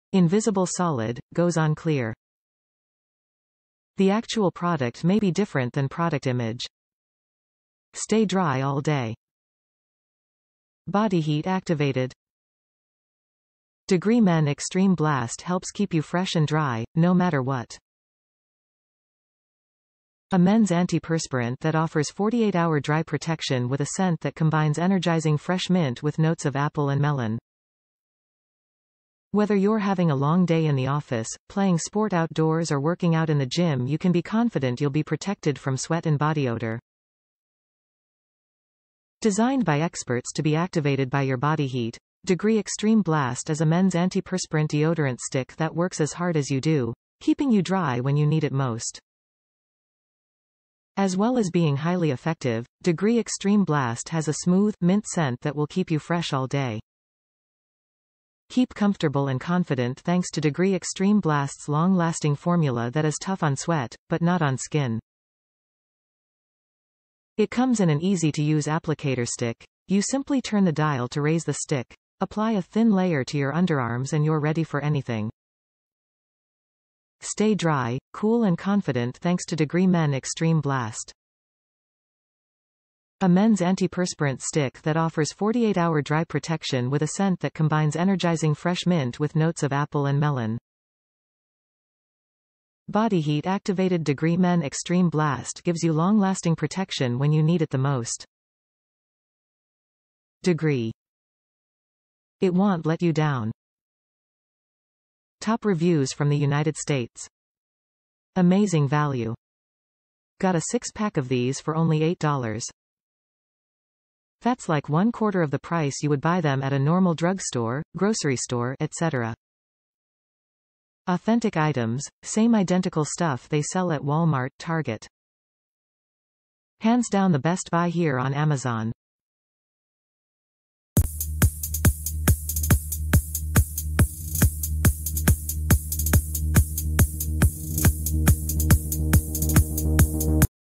Degree Men Anti-Perspirant Deodorant Invisible Stick Extreme Blast - 2.7 oz, Pack of 6
Invisible solid - goes on clear
The actual product may be different than product image
Stay dry all day
Body Heat Activated
Degree Men Extreme Blast helps keep you fresh and dry, no matter what. A mens antiperspirant that offers 48 hour dry protection with a scent that combines energizing fresh mint with notes of apple and melon. Whether youre having a long day in the office, playing sport outdoors or working out in the gym you can be confident youll be protected from sweat and body odor. Designed by experts to be activated by your body heat, Degree Extreme Blast is a mens antiperspirant deodorant stick that works as hard as you do, keeping you dry when you need it most. As well as being highly effective, Degree Extreme Blast has a smooth, mint scent that will keep you fresh all day. Keep comfortable and confident thanks to Degree Extreme Blasts long-lasting formula that is tough on sweat, but not on skin. It comes in an easy-to-use applicator stick - you simply turn the dial to raise the stick, apply a thin layer to your underarms and youre ready for anything. Stay dry, cool and confident thanks to Degree Men Extreme Blast. A mens antiperspirant stick that offers 48 hour dry protection with a scent that combines energizing fresh mint with notes of apple and melon. Body heat activated Degree Men Extreme Blast gives you long-lasting protection when you need it the most. Degree. It wont let you down.
Amazing value
Got a 6 pack of these for only $8.
Authentic items, same identical stuff they sell at Walmart/target.